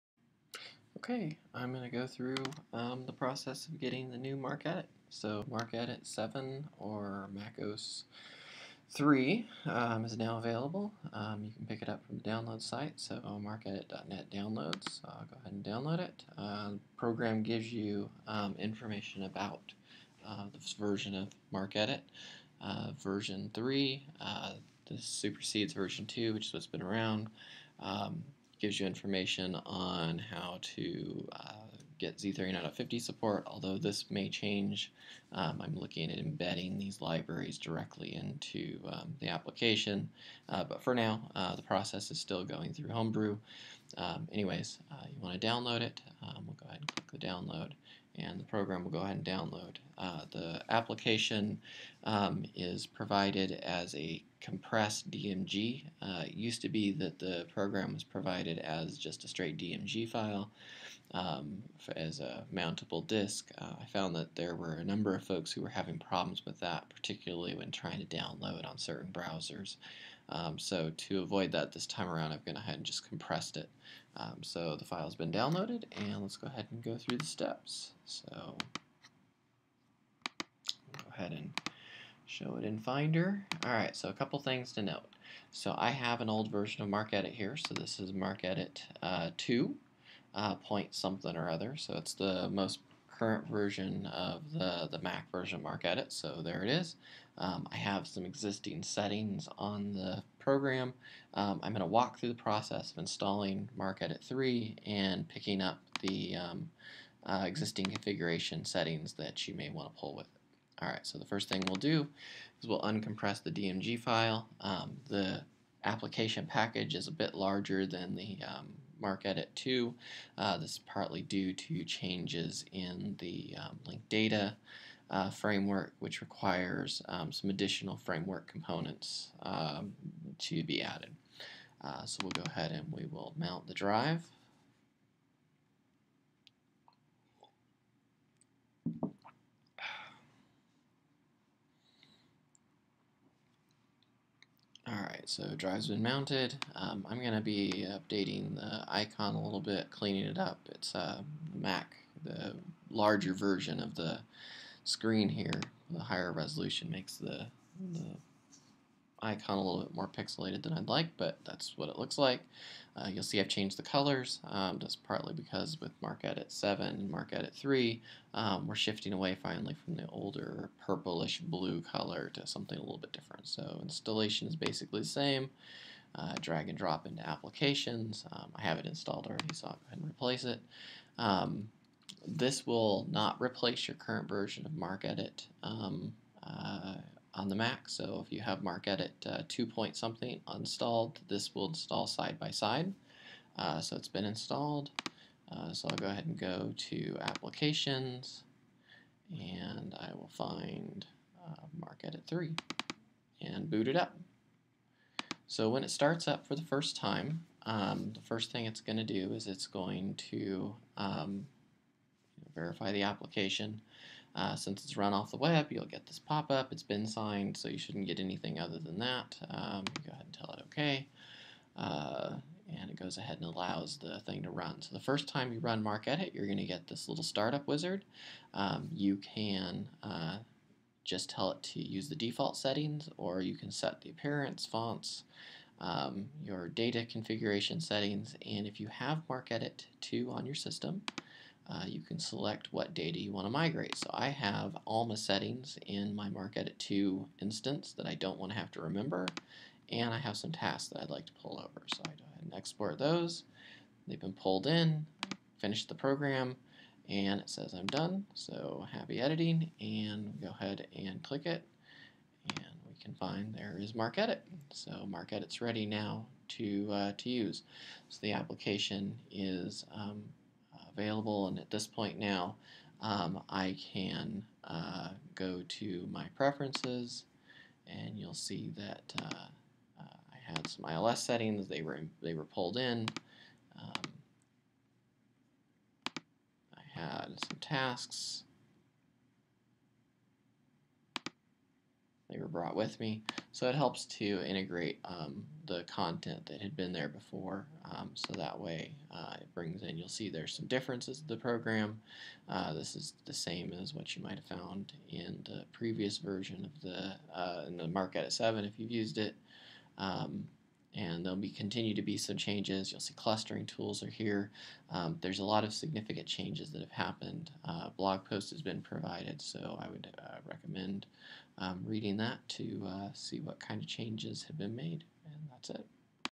MarcEdit MacOS 3: Getting Started
MarcEdit MacOS 3 is available and ready to use.  This video documents how users get it and migrate their existing data.